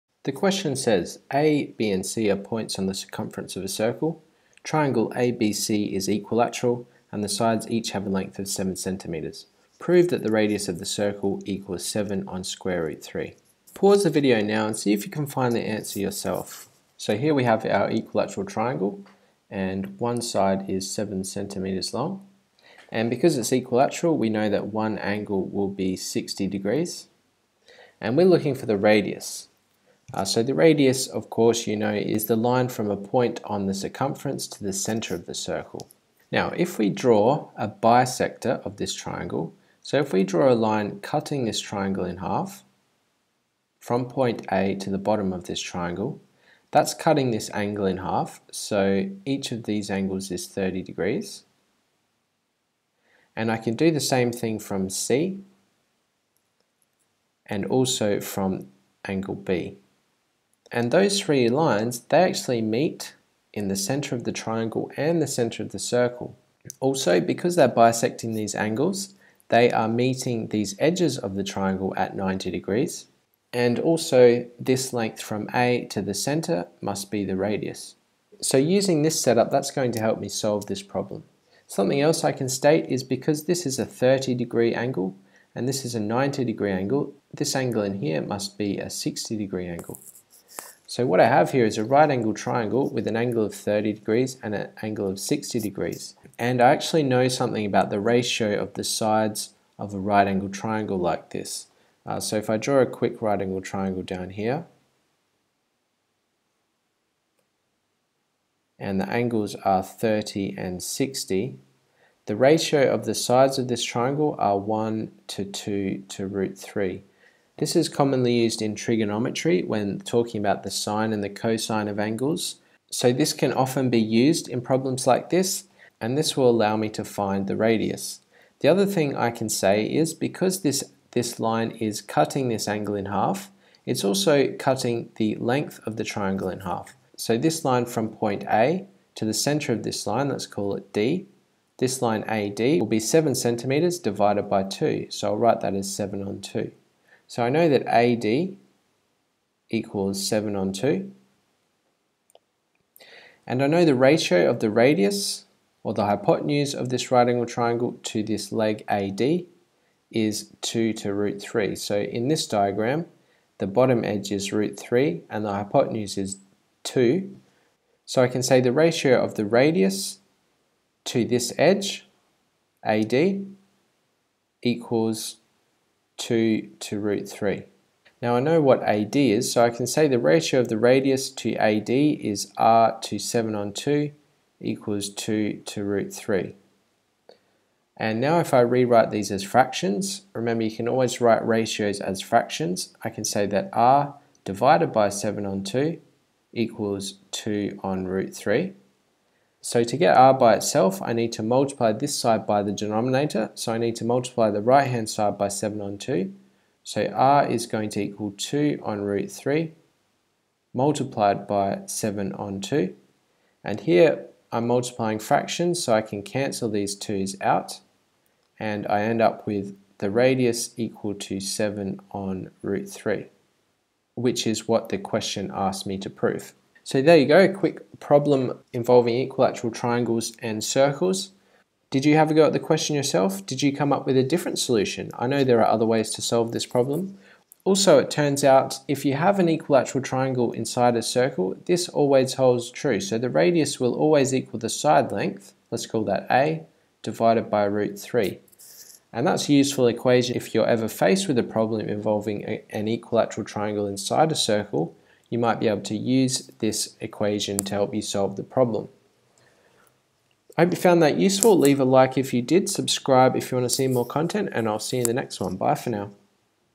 Equilateral triangle inside a circle problem
Finding the radius given the side length of a circumscribed equilateral triangle.